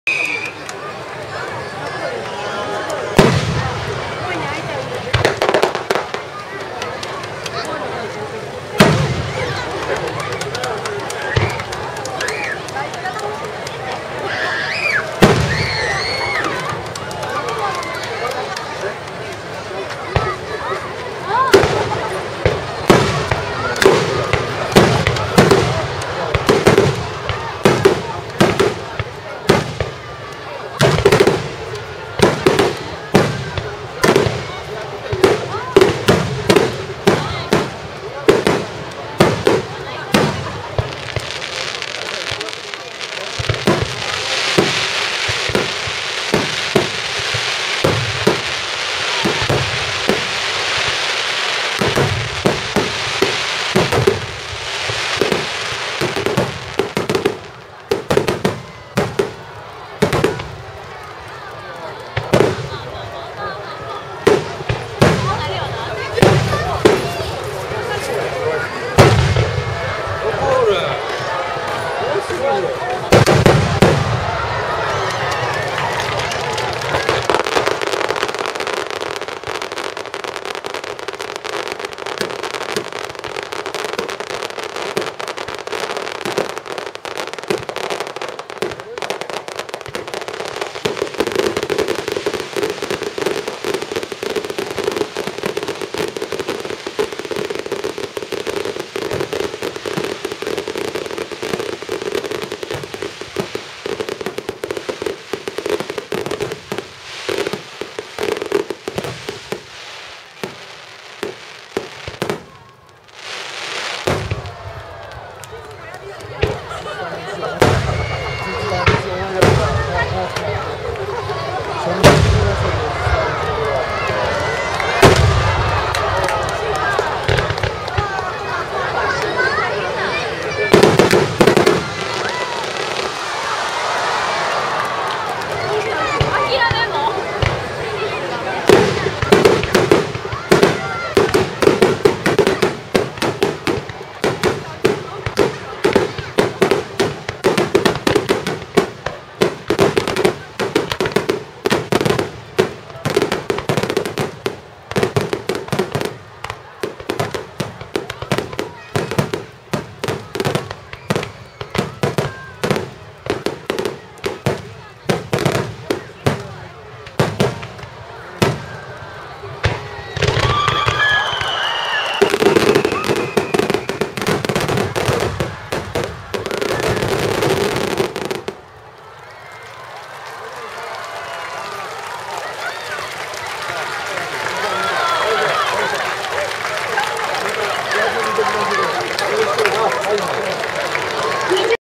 木津川夏祭り2012 花火フィナーレ.avi
木津川市の夜空を彩る3000発の花火。
その感動を再度動画にて皆様にお届けします。